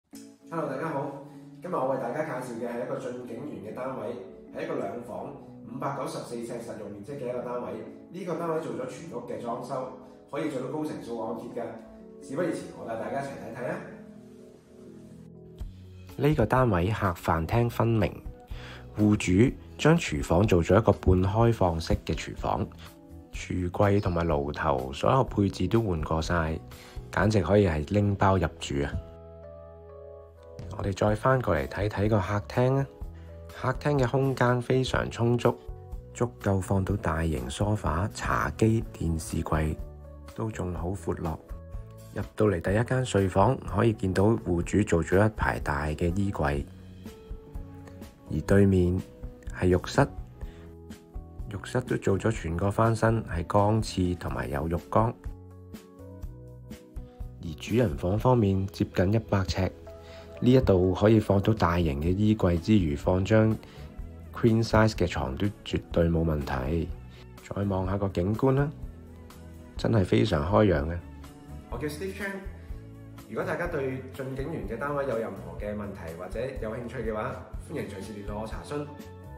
【代理Steve推介】駿景園 1期 05座 中層 C室
KOL帶睇樓 - Steve Chan（S-645547）筍盤推介：

🏠駿景園 1期 05座 中層 C室（物業編號：M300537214）

物業地址：火炭 駿景路1號
實用面積：594呎
間隔：2房
物業設施：會所、泳池、兒童設施、運動設施、娛樂設施、餐飲設施、美容／保健

屋苑：駿景園 1期 全6座 1224伙
入伙日期：13／09／1995
校網/校區：小學：91；中學：沙田
發展商：新鴻基